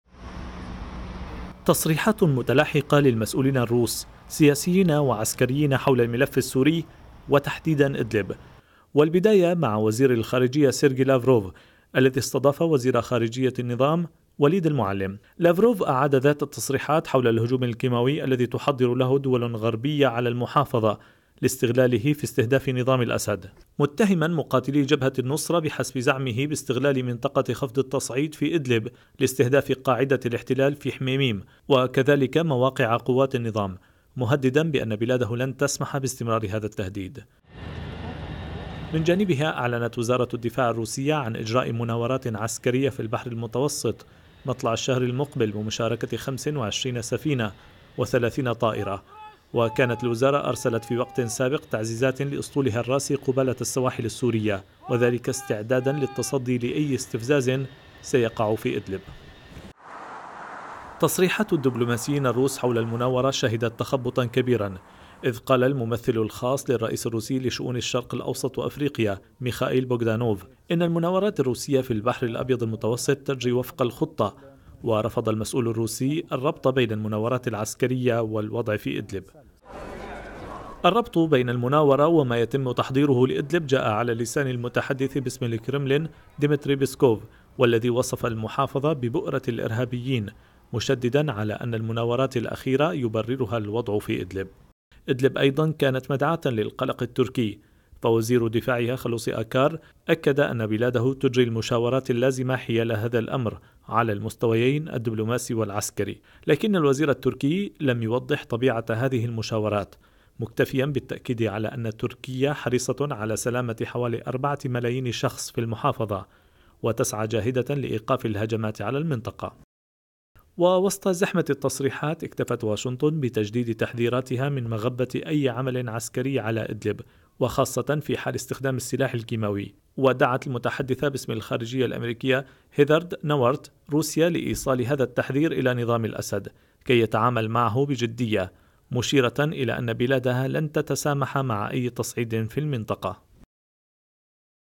سيرغي لافروف: أي عمليات غربية في إدلب "لعب بالنار"
حذر وزير الخارجية الروسي، سيرغي لافروف، الغرب من "عدم اللعب بالنار" عبر محاولة القيام باستفزازات في منطقة خفض التصعيد بمحافظة إدلب
كما أعلن لافروف، أن قيام من وصفهم بالإرهابيين باستخدام منطقة خفض التصعيد في إدلب لمهاجمة القاعدة الروسية في سوريا بالطائرات المسيرة أمر غير مقبول